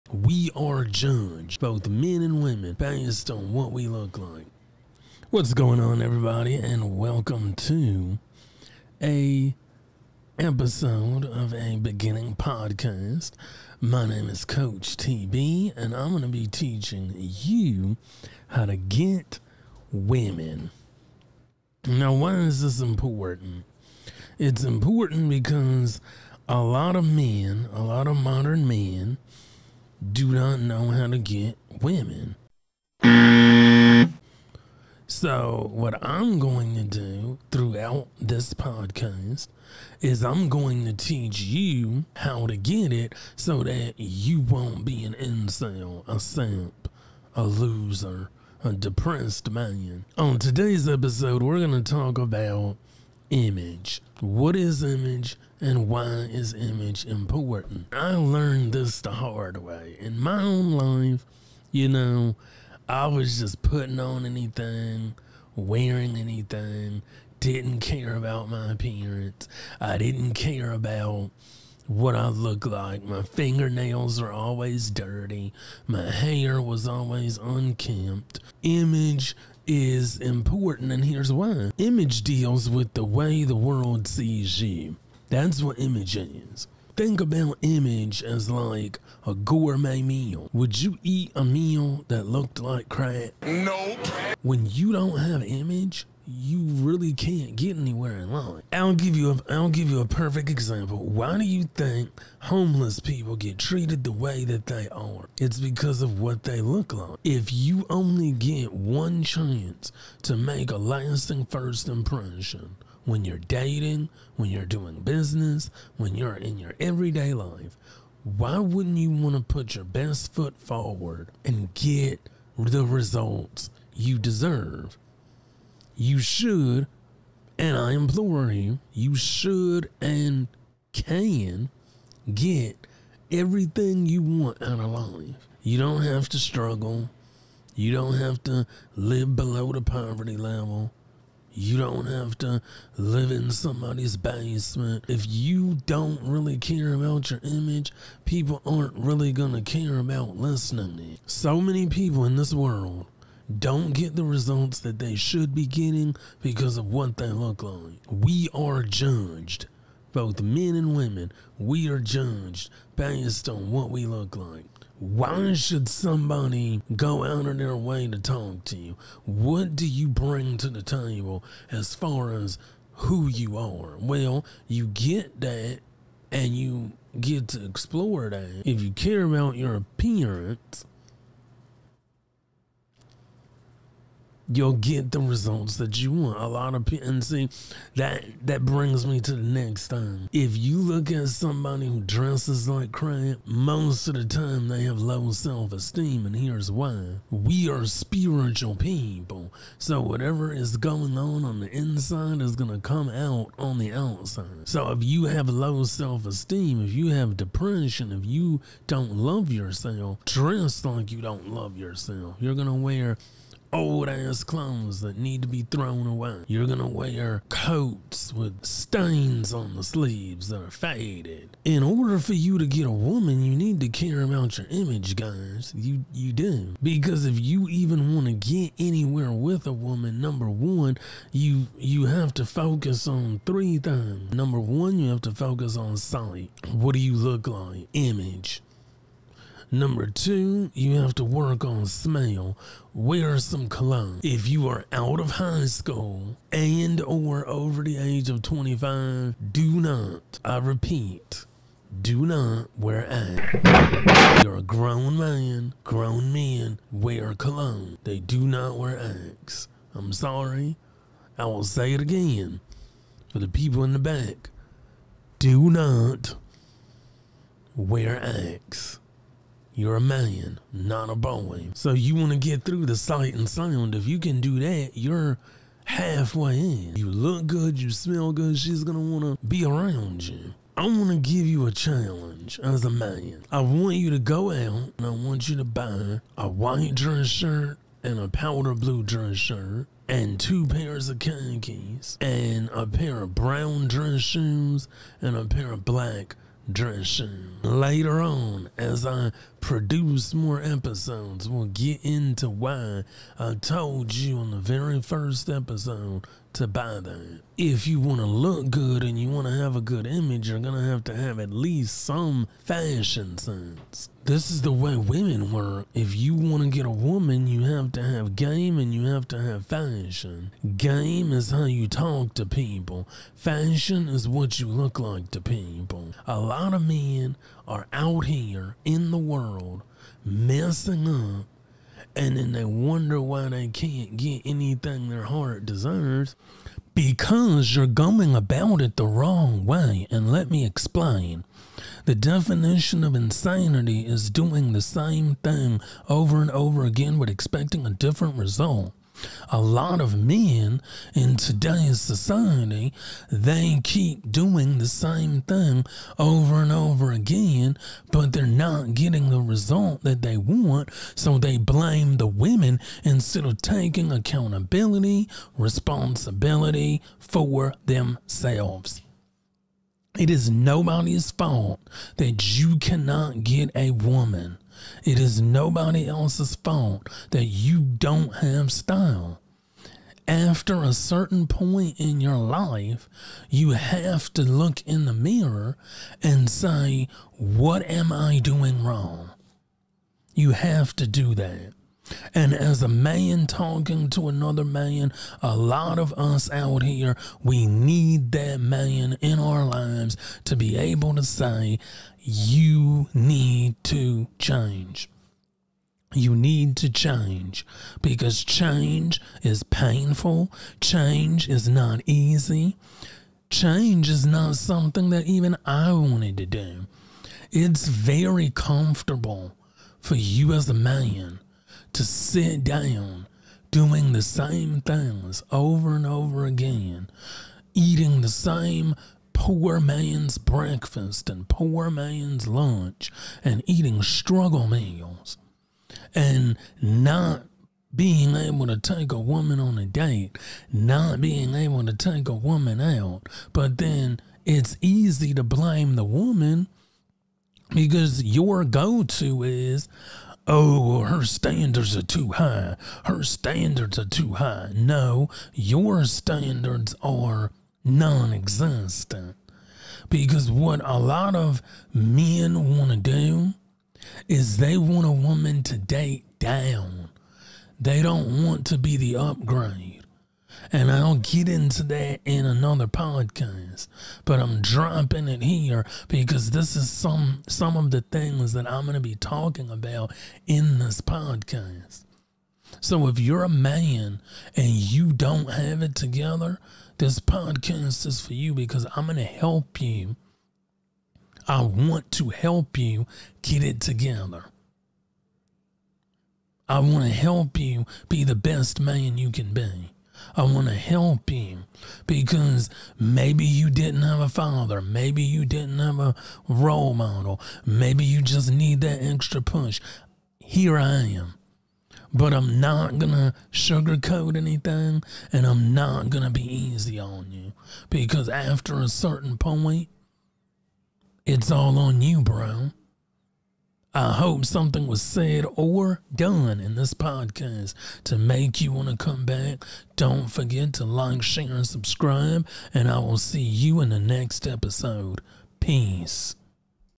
Why your image is important
Why is a person's image important? Your image/style is what defines you in society and the world. Guard your image at all costs.